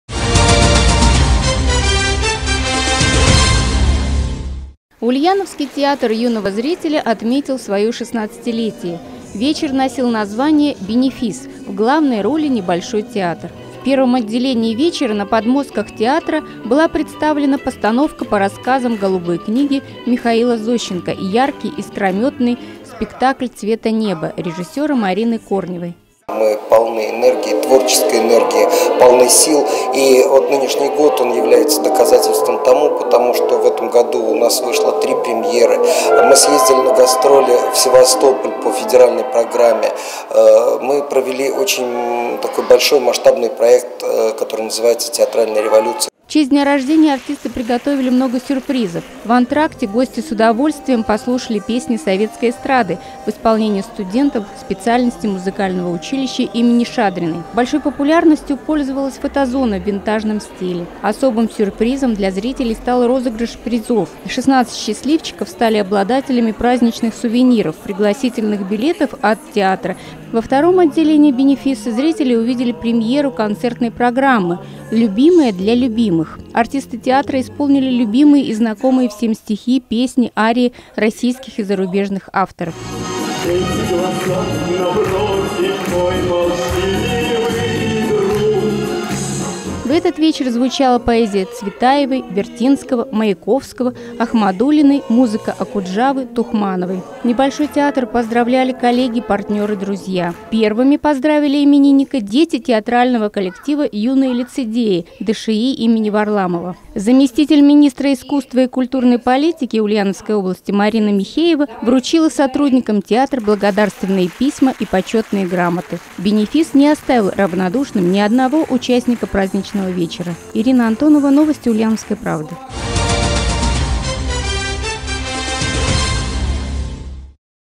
На 16-летие NEBOLSHOЙ ТЕАТР устроил вечер сюрпризов
Ульяновский ТЮЗ отпраздновал свой день рождения. «NEBOLSHOY ТЕАТР» был создан в 2001 году, и вот уже 16-й театральный сезон радует зрителей творчеством, неиссякаемой энергией, яркими премьерами и интересными проектами.